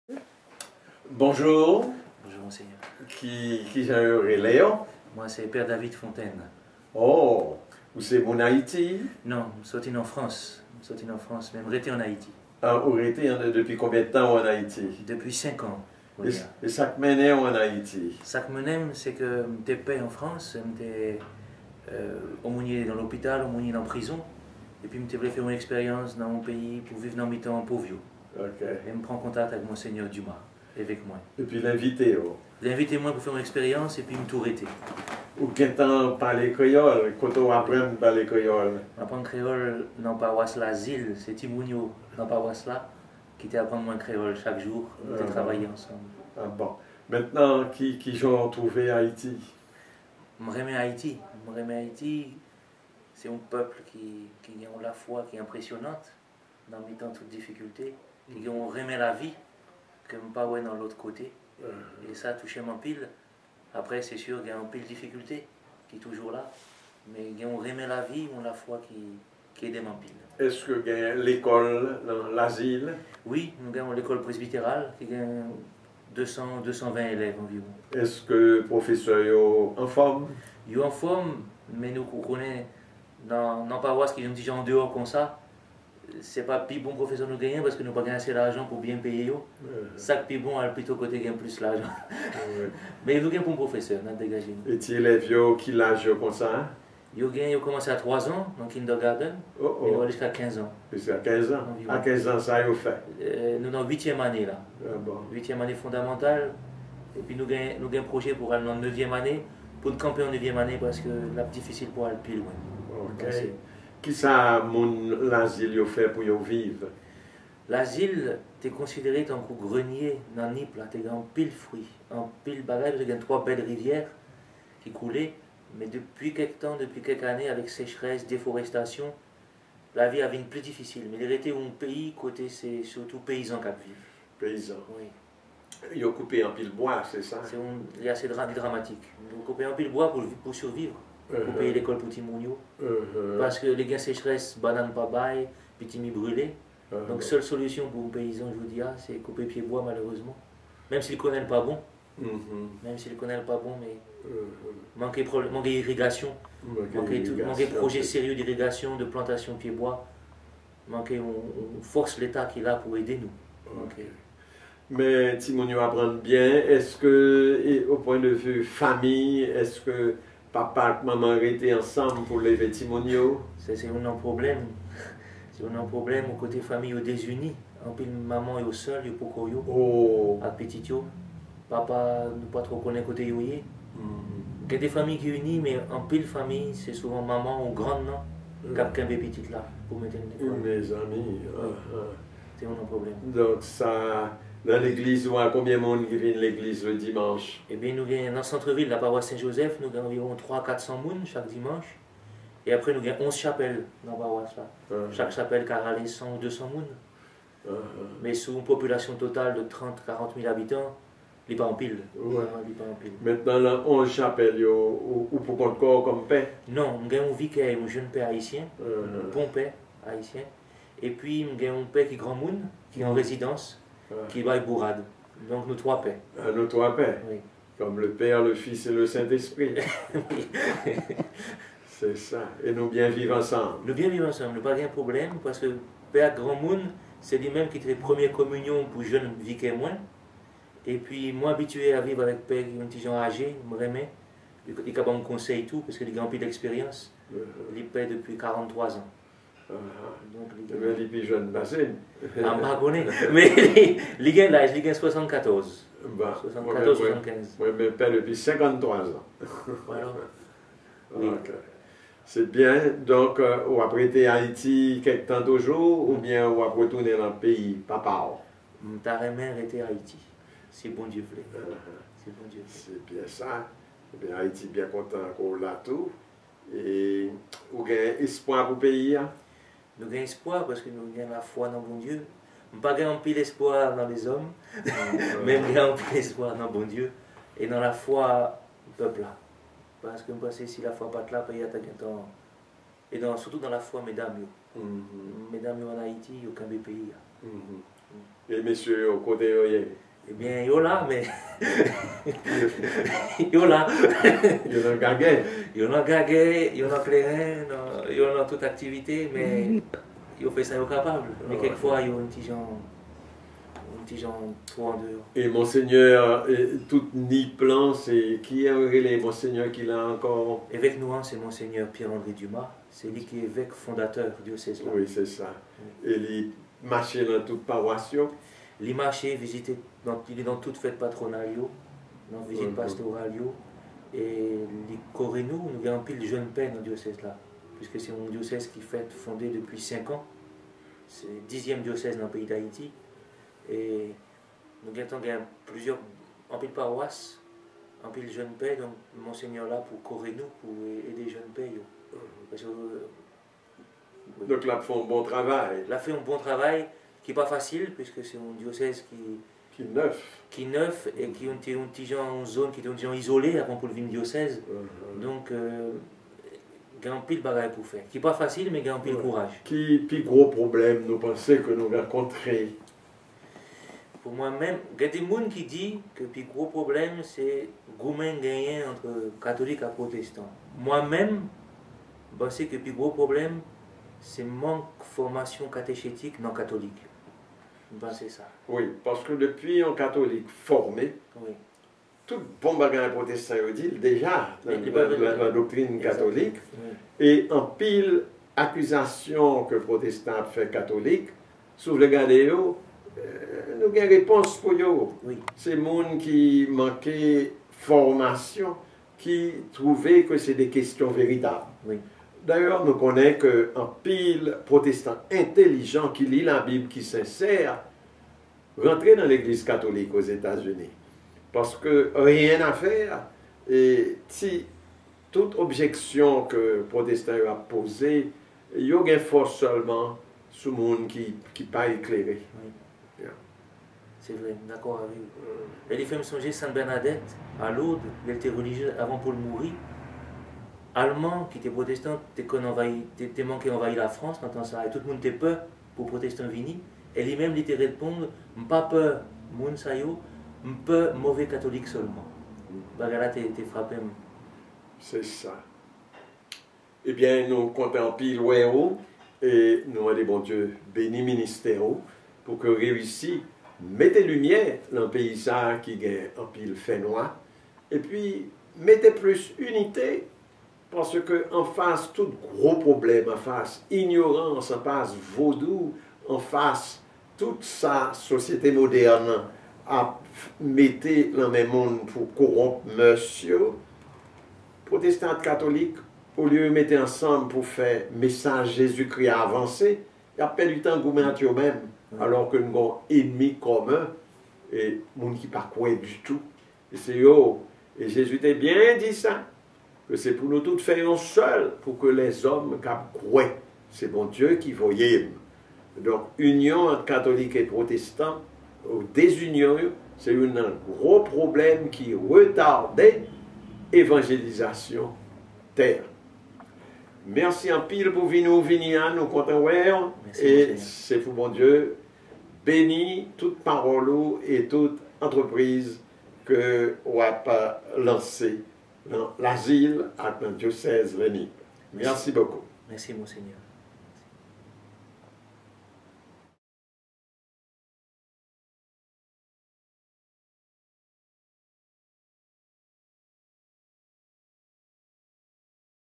Bishop G. Sansaricq interview with Msgr David Fontaine
Auxiliary Bishop Guy Sansaricq of the Haitian Apostolate in the Diocese of Brooklyn interviewed Msgr. David Fontaine of the Diocese of Nippes, Haiti on March 26, 2014.